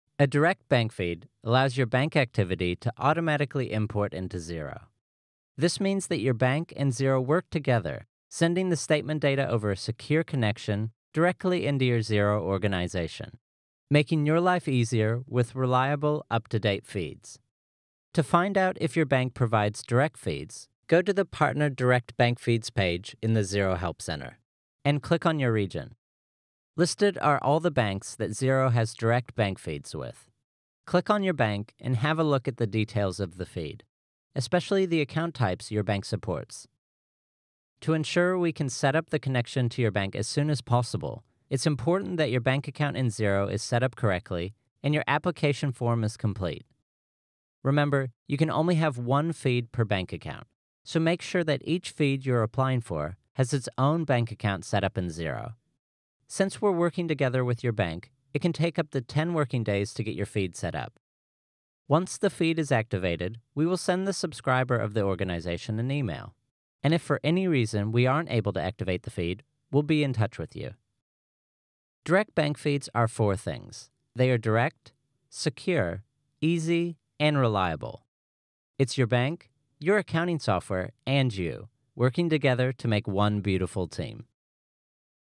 Introduction to bank feeds in Xero
A direct bank feed allows your bank activity to automatically import into Xero. This means your bank and Xero work together, sending the statement data over a secure connection directly into your Xero organisation. Making your life easier, with reliable and up to date feeds.

To find out if your bank provides direct feeds go to the 'Direct Bank Feeds' page in the Xero Help Centre, and click on your region. Listed are all of the banks Xero has direct feeds with. Click on your bank and have a look at the details of the feed, especially the account types your bank supports.